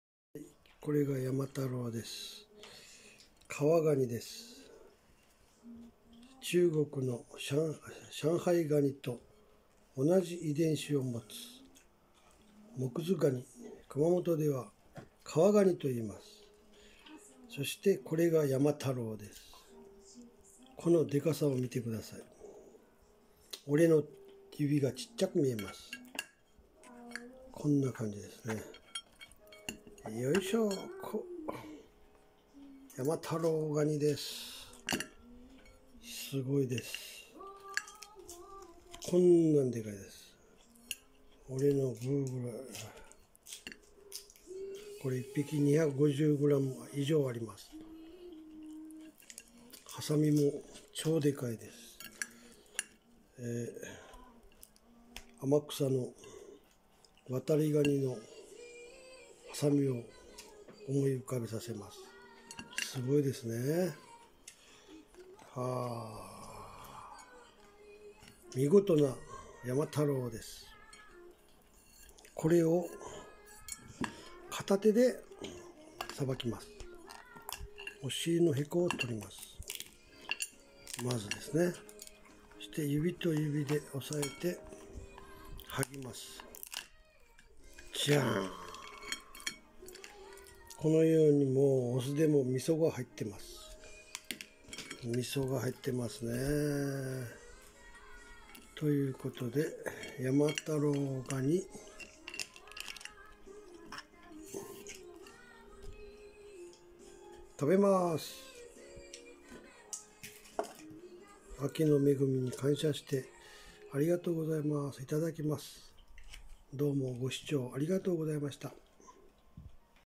🦀山太郎ガニを食う🦀
🦀山太郎ガニが😂３匹獲れました🦀１匹２５０グラム有りますよ👏超美味しいですよ😋オスのハサミの身が超美味しいです😃